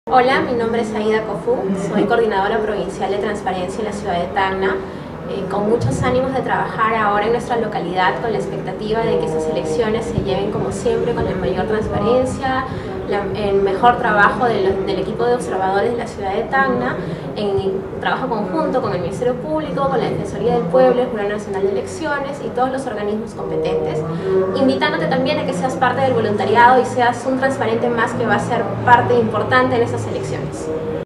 Hazte un Transparente! Únete a nuestra red de voluntarios de Transparencia
Aida Cofú, coordinadora provincial en la región de Tacna.

Únete a Transparencia.